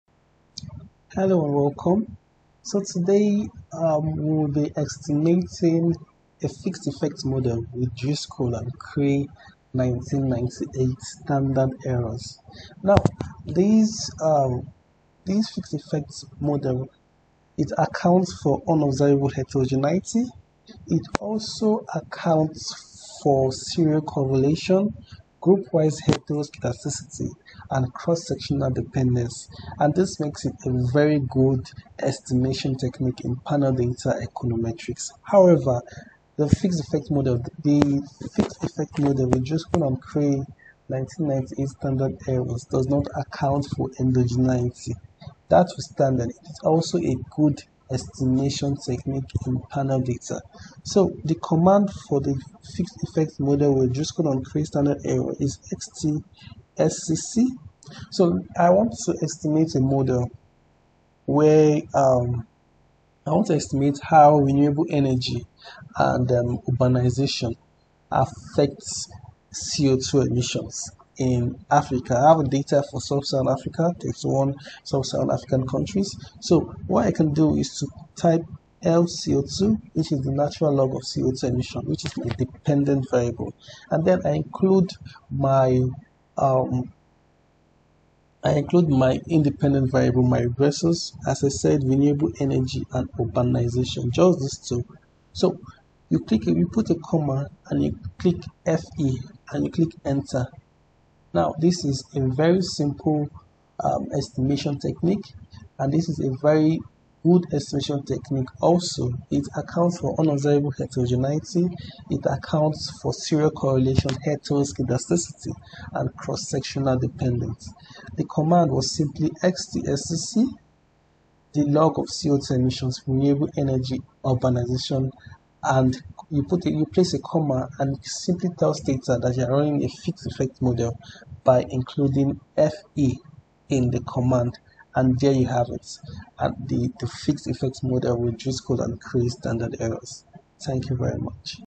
Fixed Effect (FE) Model with Driscoll and Kraay Standard Errors STATA
The FE Driscoll and Kraay Model accounts for Serial Correlation, Group Wise Heteroskedasticity and Cross-sectional Dependence.